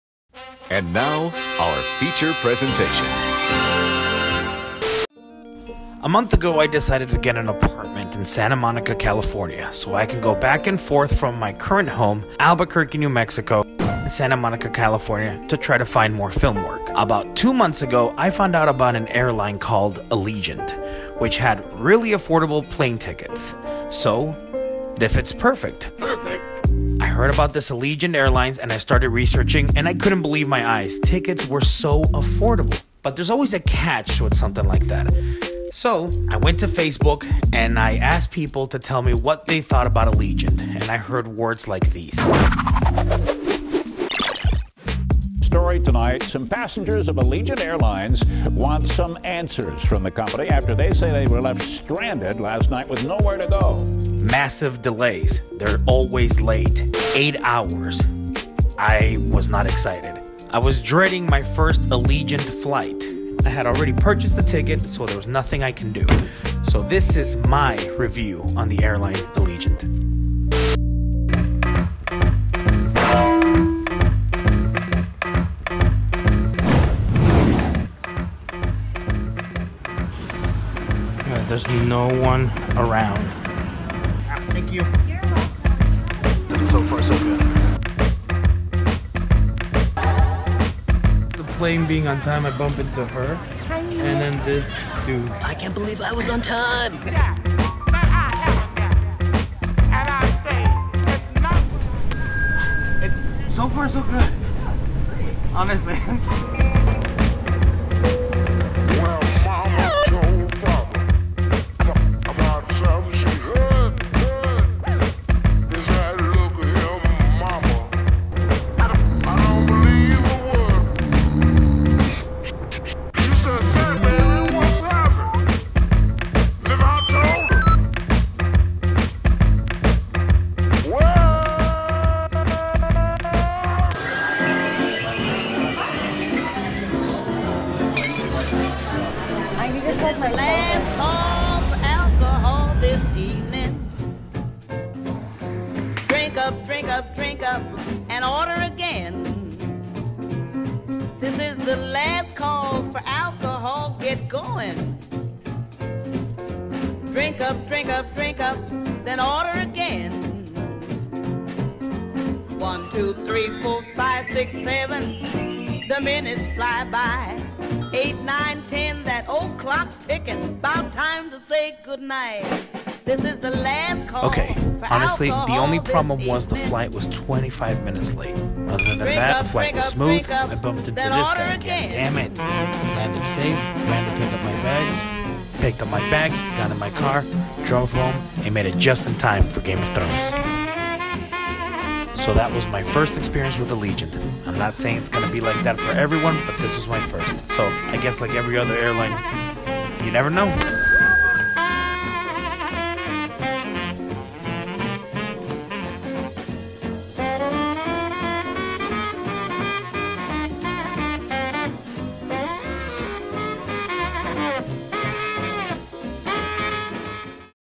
Allegiant Airlines, is it worth it?
Is Allegiant as bad as they say it is? This is my first time flying with them.

Shot on I phone 7
Drone shots  : @neilvshootsraw
Special guests: @rebarbara06 and @rustarhymes
Art  : @rokrjon
Edited and directed : @alejandromontoyamarin